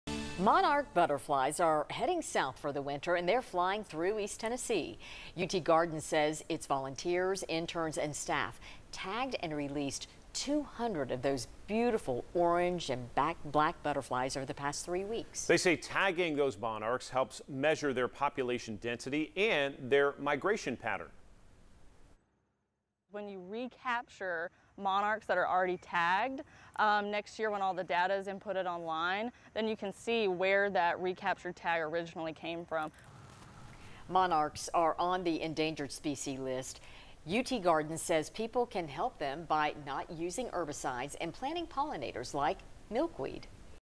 UT Gardens tags and releases 200 butterflies for population density and migration research
The tags are lightweight stickers designed to be harmless to butterfly wings.